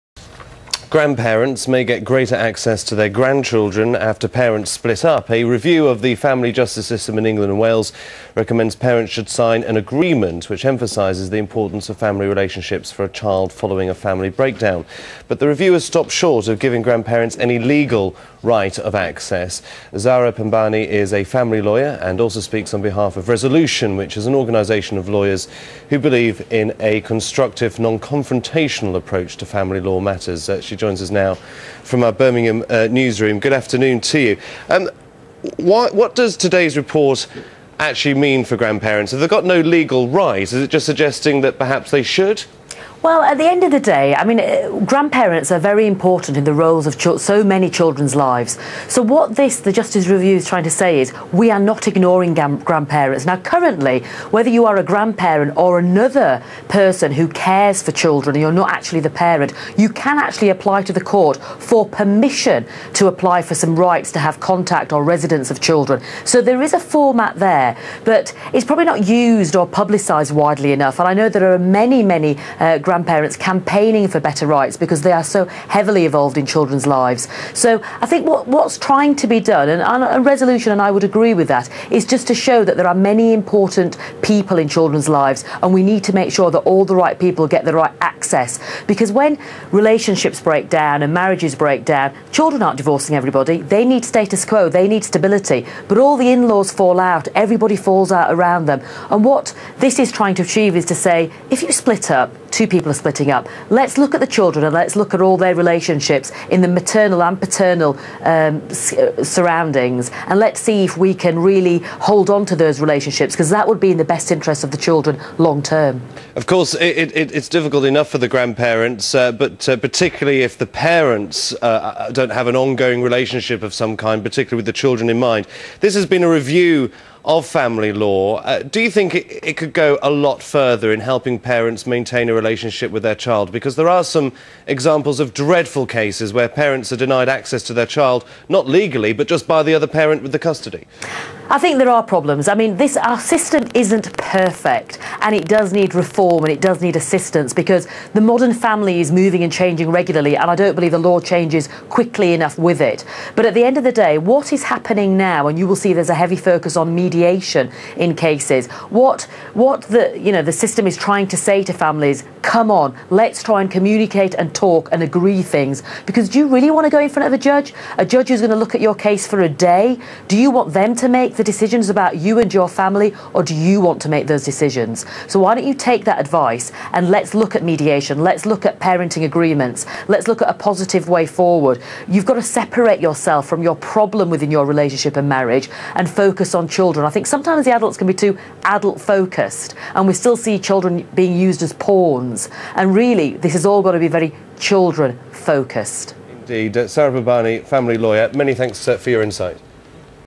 Grandparents' and Children's Rights
Resolution member Zahra Pabini talks to BBC News about grandparents' access rights and reforms to the family justice system. Broadcast live on 31 March 2011.